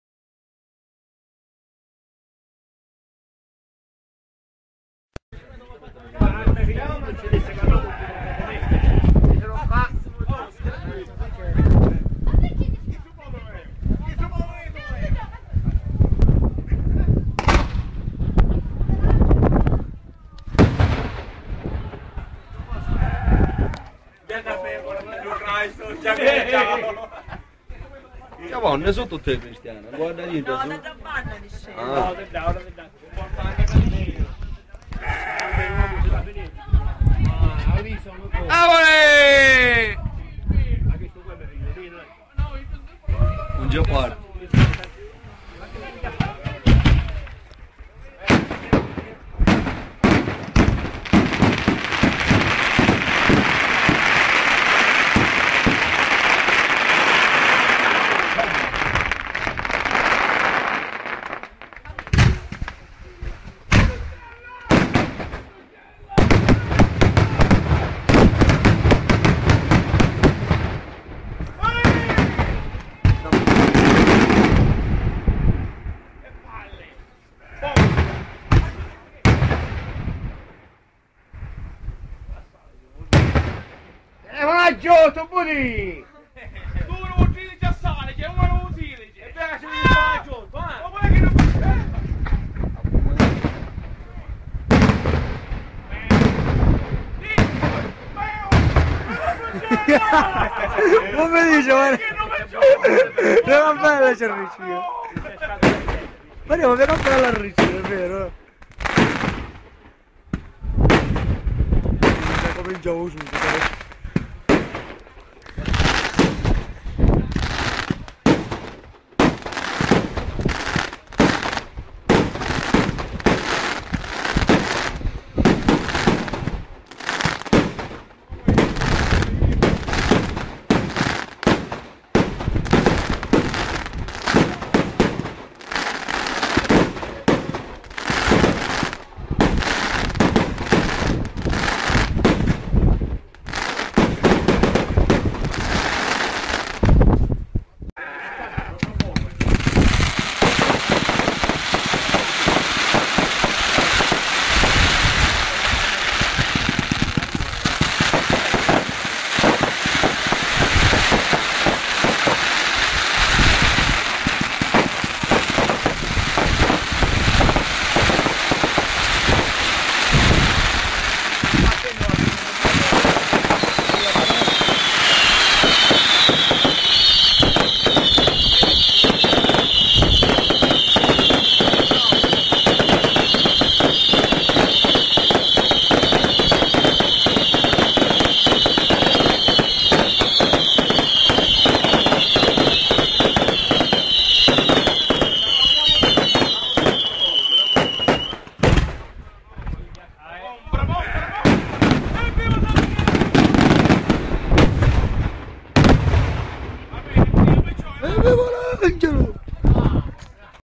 villafranca sicula 2010
spettacolo pirotecnico curato dalla BOMBALUNGA&CO per il 18° compleanno di CATERINA GIARRATANO a lucca sicula